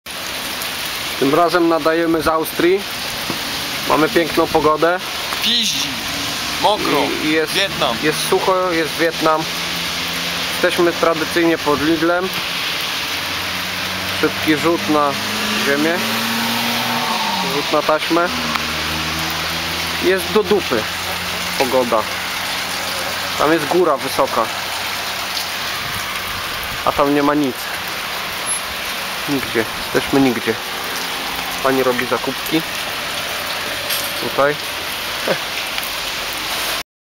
2012 Trip - Rumunia i reszta Europy część 4
Jeden z filmików nakręconych podczas podróży po Europie. Przejechaliśmy 6600km w 3 tygodnie, byliśmy w 11 krajach, a wszystko to udało się zrealizować śmiesznie niskim kosztem. Blog z podróży, zdjęcia itp. znajdują się na